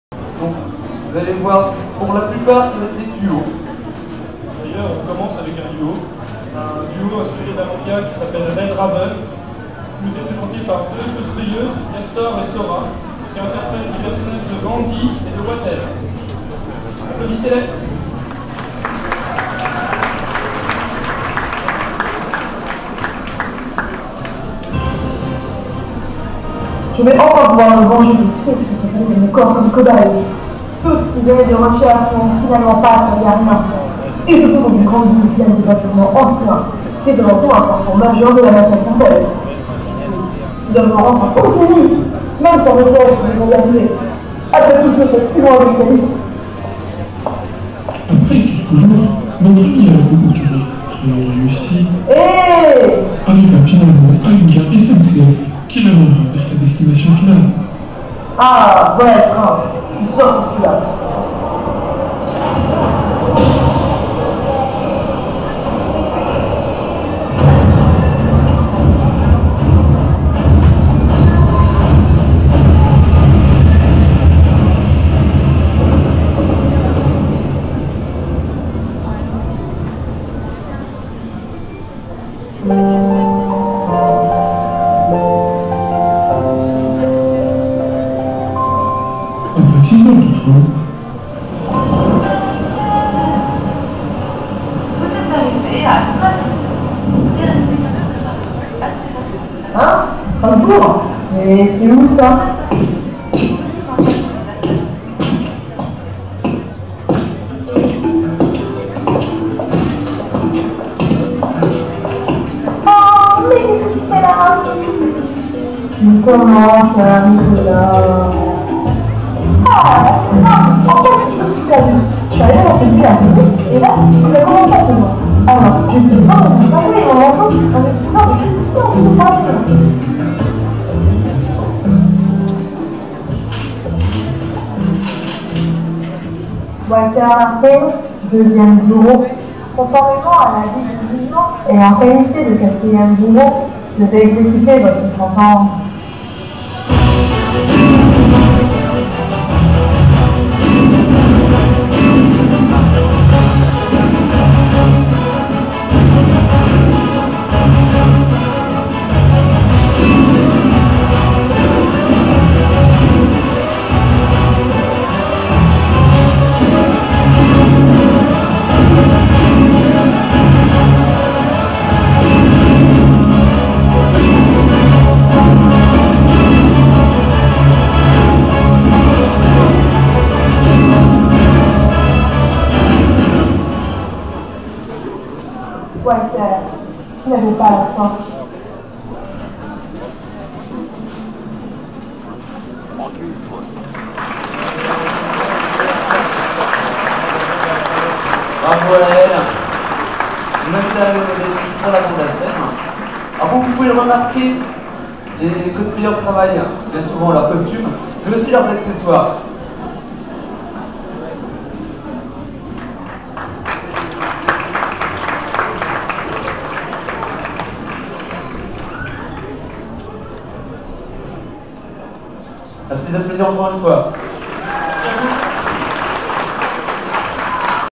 Jap and Co 2013 - Concours Cosplay - Duo Red Raven
Voici le duo Red Raven qui est passé lors du concours cosplay de la Jap and Co.